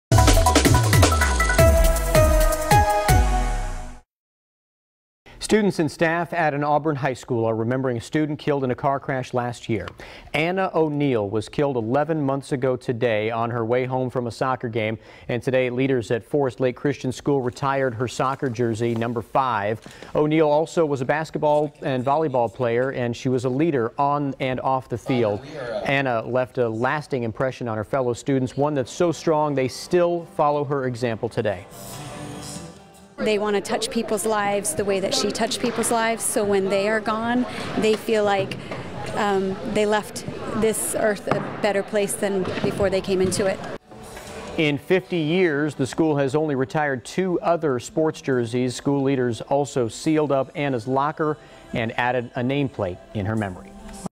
Auburn school retires beloved student’s athletic jersey
Forest Lake Christian School held a jersey retirement ceremony Wednesday for Ann O’Neill who was killed 11 months ago in a car accident while on her way home with her family from a school league soccer game.